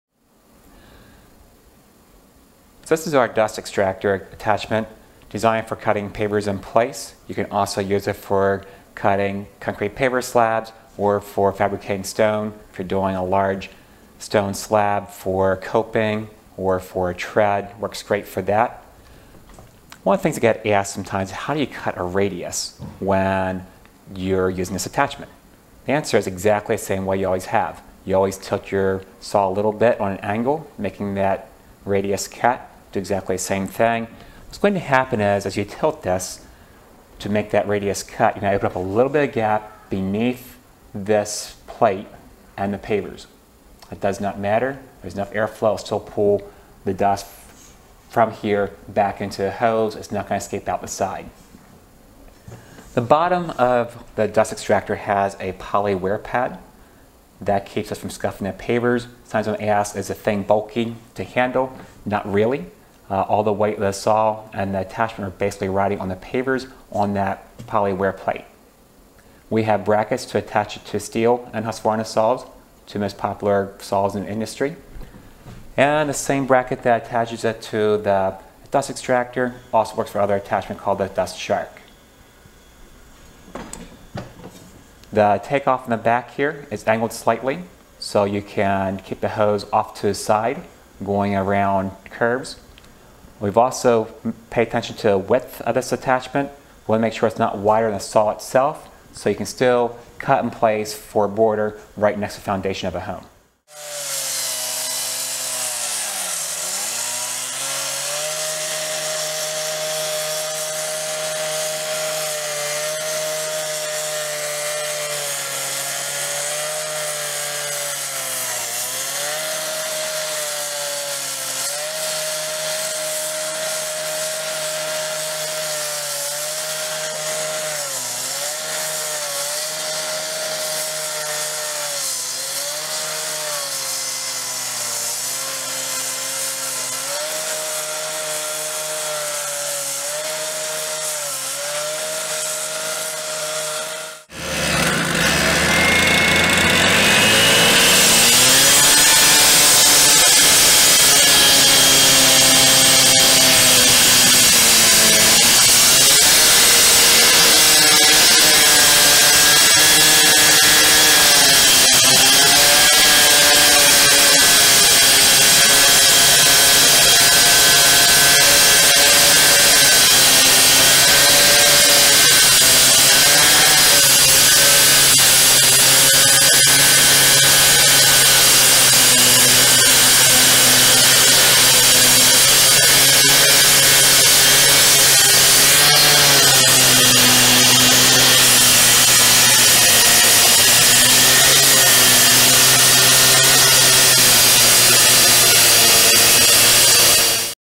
Dust Xtractor- How to collect dust when cutting pavers in place
The Dust Xtractor is designed to collect dust while cutting pavers in place, or when cutting relief joints into concrete slabs.  It is also effective when cutting large stone slabs for countertops or treads.

The Dust Xtractor can be attached to Husqvarna or Stihl handheld saws.